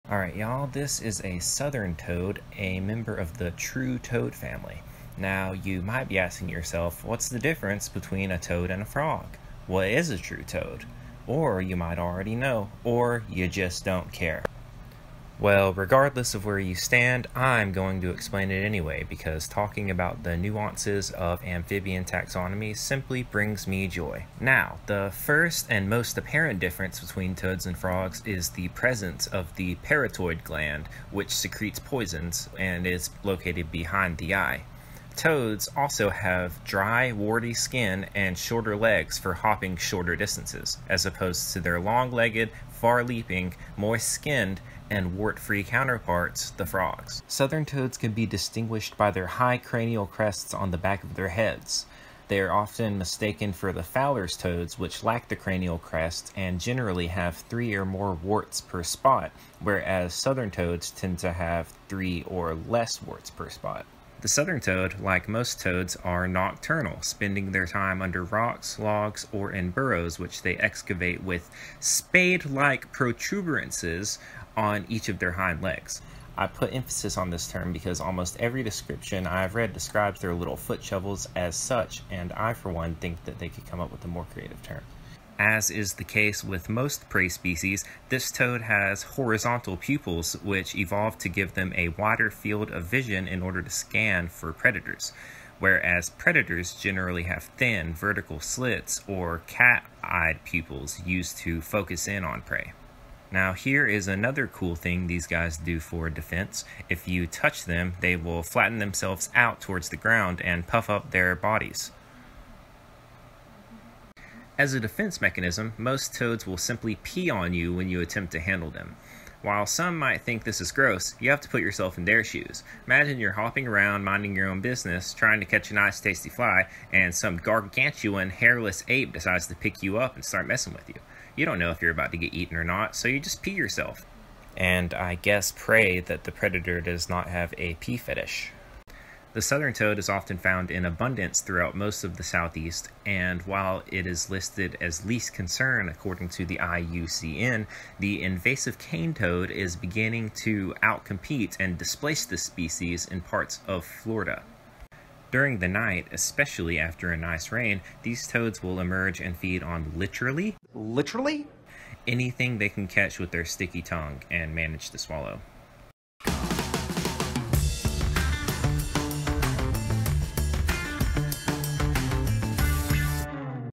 Why Toads Pee On You When You Pick Them Up (Southern Toad)
The Southern Toad has a few interesting defense mechanisms it will employ to deter predators. Have you ever wondered why toads pee on you when you pick them up? It's a defense mechanism! In this video, I also go over the differences of frogs vs toads. One of the key differences is that toads have a parotoid gland, or poison gland, located behind the eye. Toads also have dry skin with warty skin. Despite this, it is actually a myth that toads cause warts.

Southern Toad (Anaxyrus (Bufo) terrestris) description:
The average body length of southern toads is 41-75 mm (1.6-2.9 in), although some individuals found on isolated island populations can reach 113 mm (4.4 in). Males are smaller than females, and typically have a darker throat during the breeding season. General coloration is usually brown but can vary to dark gray, blackish, or even brick red. Several dark spots or blotches are frequently present on the back and upper sides, each surrounding one or more warts. A light line extends partway down the middorsal of some southern toads, although it may be obscure or absent in others. The belly is grayish white, while the chest is spotted. The skin of these toads is dry and warty, and they have an elongated, enlarged parotoid gland behind each eye. High cranial crests extend from pronounced knobs and approach each other toward the snout. The distinctive knobs project upward in front of the parotoid glands, often giving the animal a horned appearance.